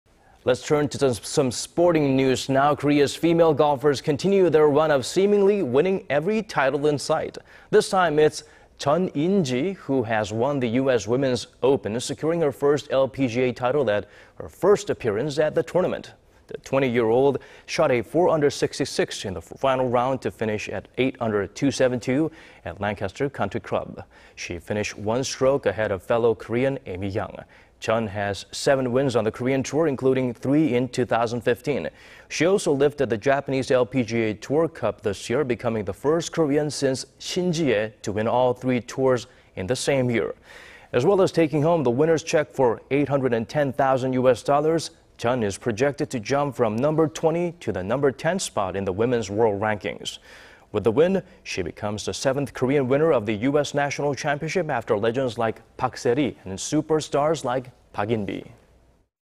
Korea′s Chun In－gee wins U.S. Women′s Open 전인지， US여자오픈골프 역전승…한미일 동시 제패
Korea′s female golfers continue their run of seemingly winning every title in sight.
This time it′s Chun In－gee... who has won the U.S. Women′s Open， securing her FIRST LPGA title at her FIRST appearance at the tournament.
The 20－year－old shot a 4－under－ 66 in the final round to finish at 8－under 272 …at Lancaster Country Club.
She finished one－stroke ahead of fellow Korean Amy Yang.
Chun has seven wins on the Korean tour， including three in 2015.
She also lifted the Japanese LPGA Tour cup this year， becoming the first Korean since Shin Ji－yai... to win all three tours in the same year.
As well as taking home the winner′s check for 810－thousand U.S. dollars， Chun is projected to jump from number 20 to the number 10 spot in the women′s world rankings.
With he win， she becomes the seventh Korean winner of the U.S. national championship after legends like Park Se－ri... and superstar Park In－bee.